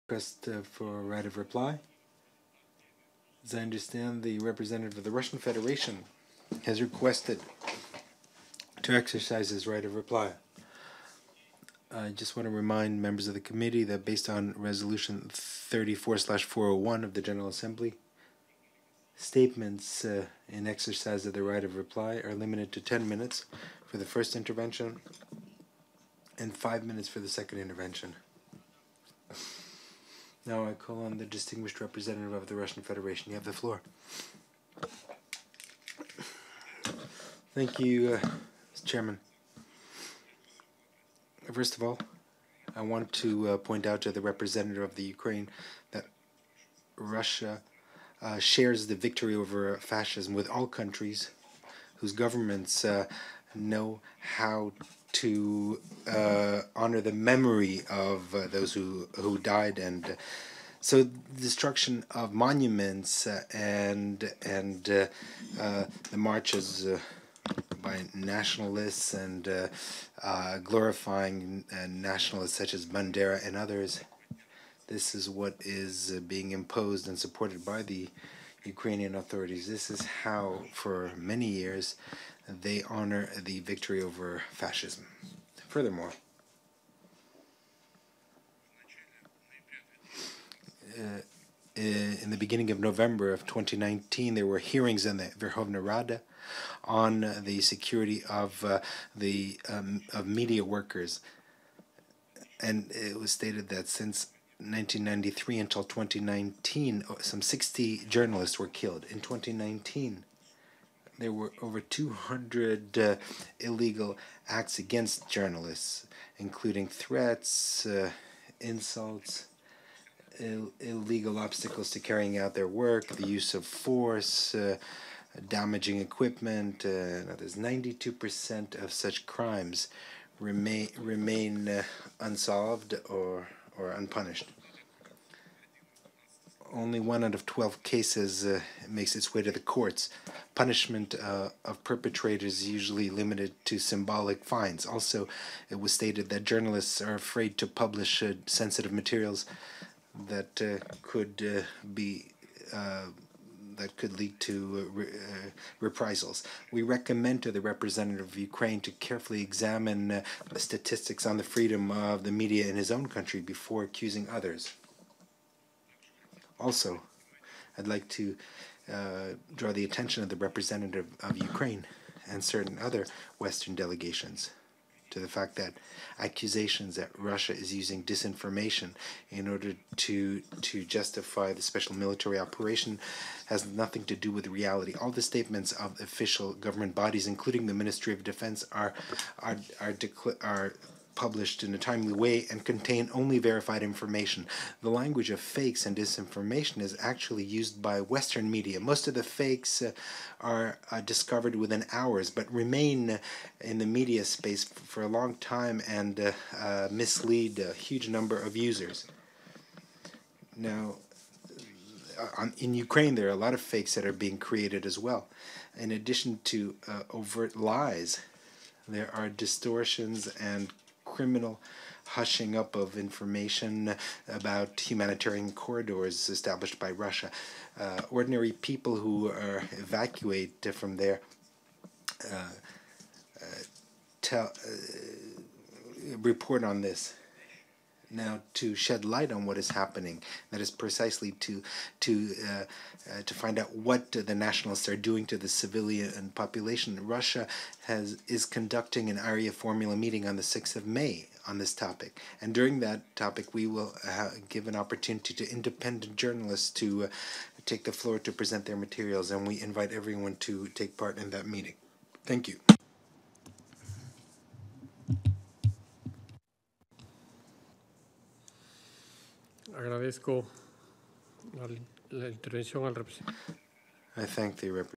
Right of reply by Mr. Fedor Strzhizhovskiy after the statement by the representative of Ukraine
Right of reply by the representative of the Russian Federation Mr. Fedor Strzhizhovskiy after the statement by the representative of Ukraine at the General Debate of the 44th Session of the Committee on Information (COI), May 4, 2022